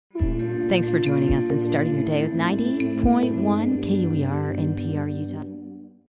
KUER 90.1 | NPR Utah - Thanks for Choosing Us
Staying informed is an active decision and find a source you can trust has never been harder or more important. We're thankful for those who choose KUER 90.1 | NPR Utah to start their day.